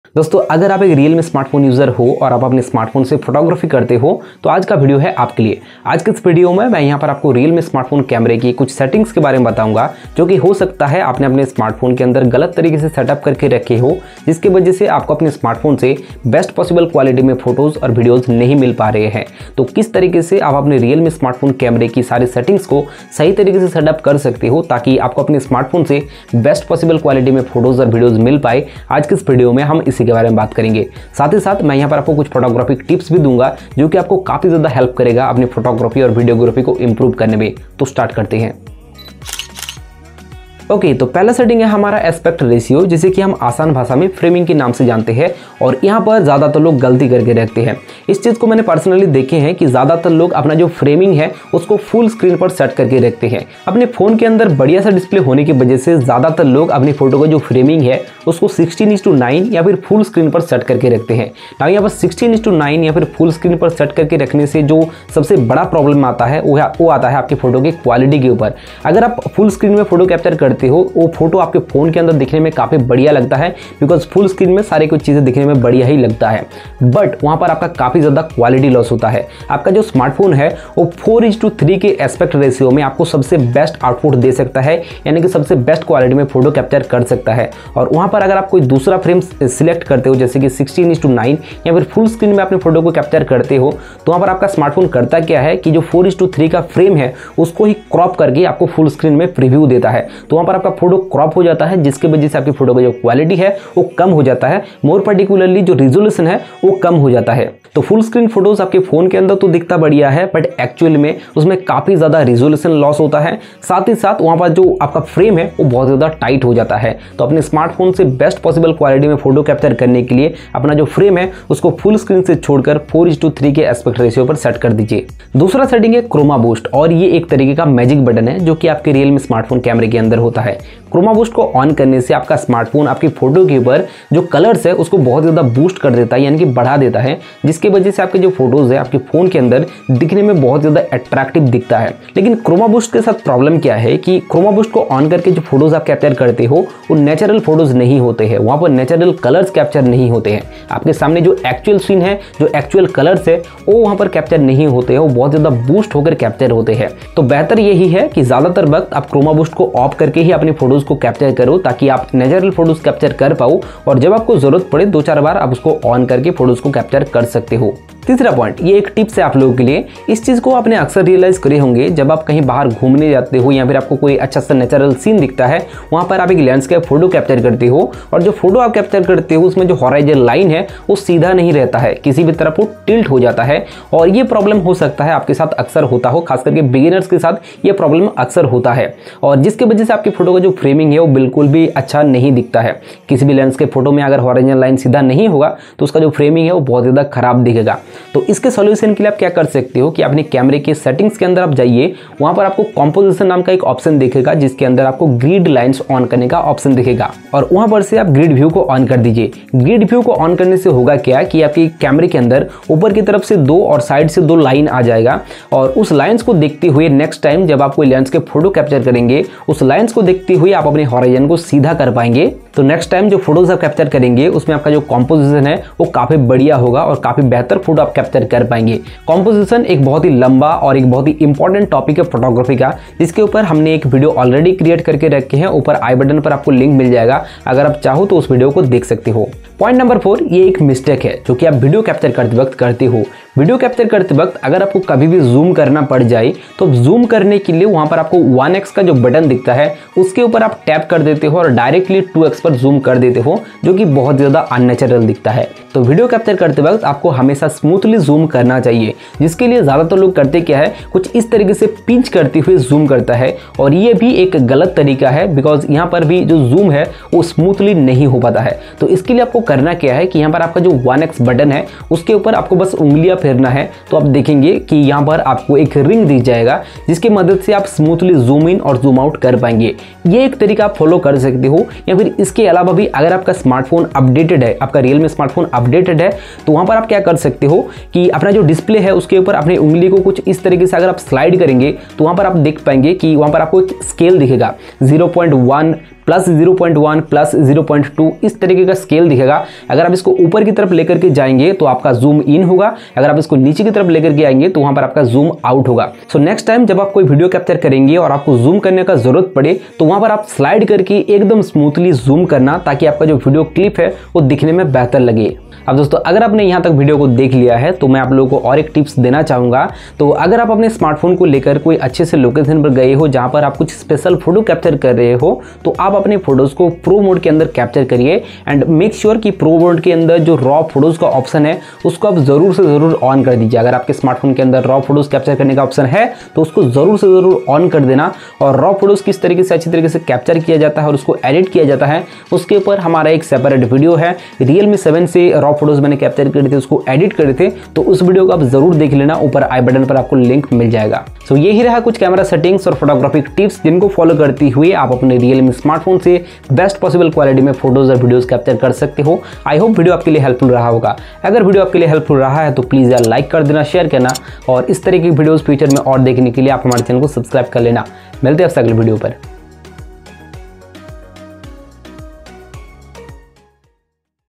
Best Camera Settings for Realme । Get best quality Photo or Video from any Realme Smartphone
Best Camera Settings for Realme 7 । How to get best quality Photo or Video from any Realme Smartphone.

GEARS I USE:

🔸Prime Lens
🔸Zoom Lens
🔸Mini Tripod
🔸LAV Microphone
🔸USB Microphone
🔸Flashlight for Camera
🔸Green Screen